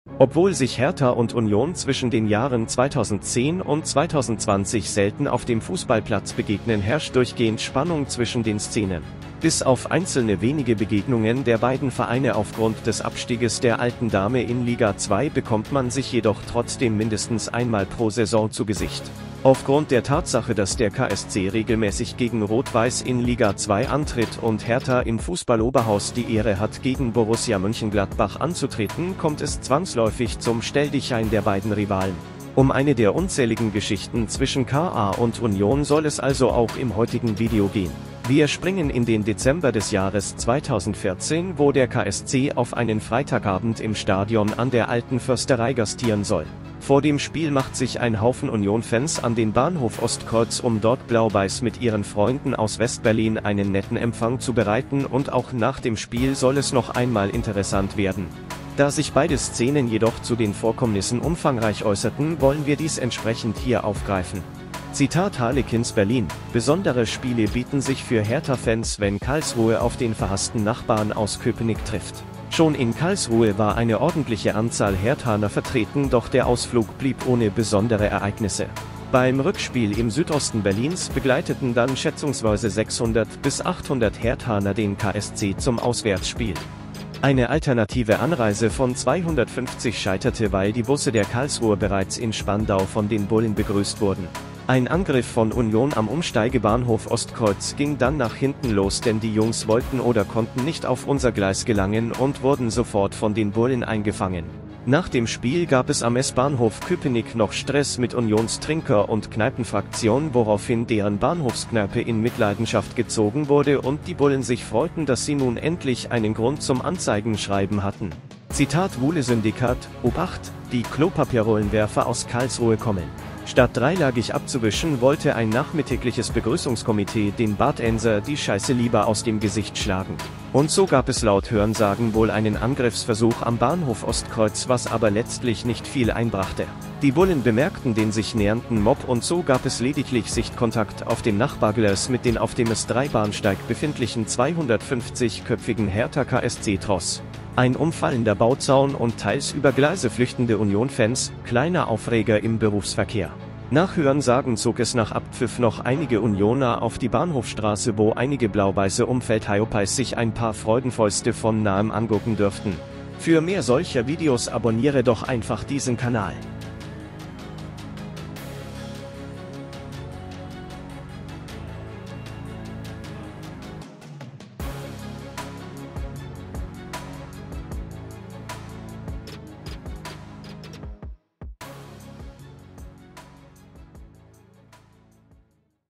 Angriff am Bahnhof Berlin-Ostkreuz! (Union vs. Hertha/ Karlsruhe)
Wir springen zurück in die Saison 2014/2015, wo der KSC mal wieder am Stadion an der Alten Försterei gastiert. Das nehmen auch einige unbelehrbare Unionfans zum Anlass, vor dem Spiel den einfahrenden Gäste-Mob mit Freunden der Hertha am Bahnhof-Ostkreuz zu attackieren...